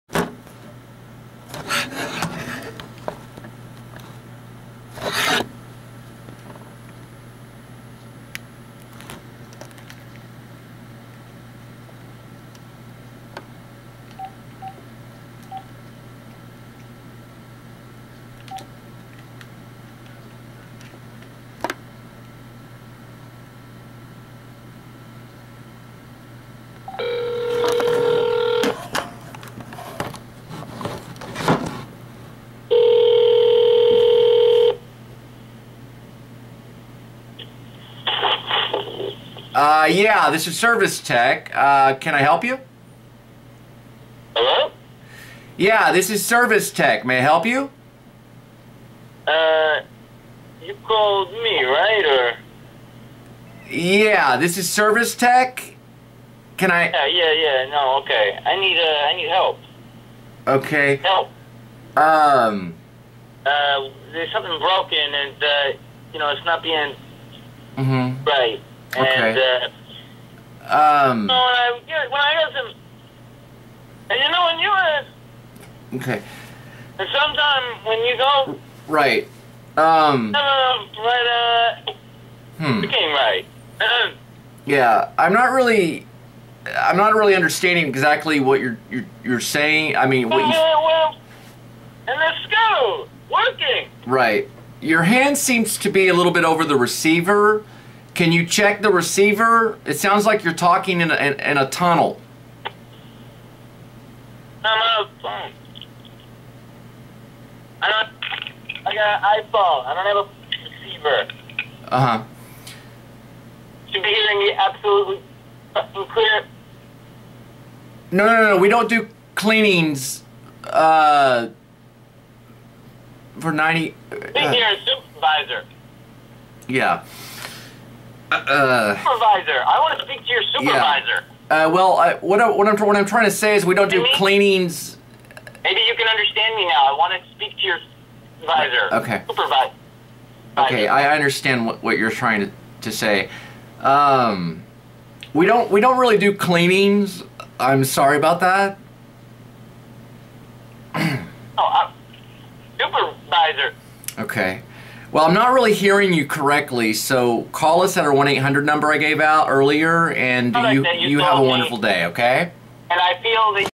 BULB (Episode 232)
John waits for call at Home Office for Service Tech. Daily 232.